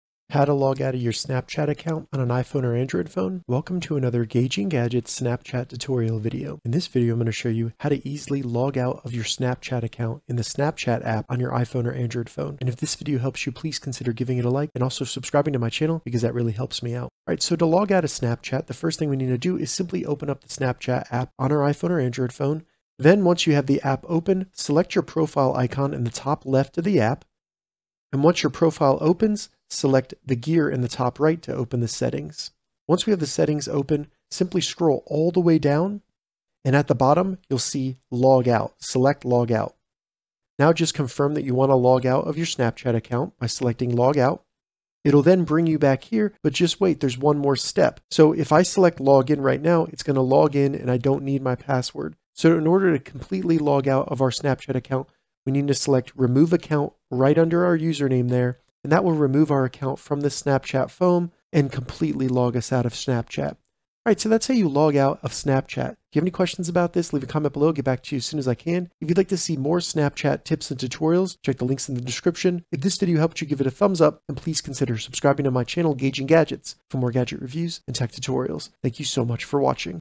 How to Logout of Snapchat app on iPhone & Android
How to logout of Snapchat app? In this tutorial, I show you how to logout of the Snapchat app on your iPhone or Android phone. Signing out of Snapchat is easy and will allow you to completely remove your Snapchat account from a phone. You can also logout of Snapchat in order to login to another Snapchat account.

Follow the steps below to send a picture as a normal snap:
1. Open the Snapchat app on your iPhone or Android phone.
2. Select the Profile Icon in the bottom menu of Snapchat.
3. Select the Gear Icon.
4. Scroll all the way down to the bottom and select Logout.
5. Confirm you want to logout of Snapchat.
6. To completely remove your account from Snapchat select Remove Account.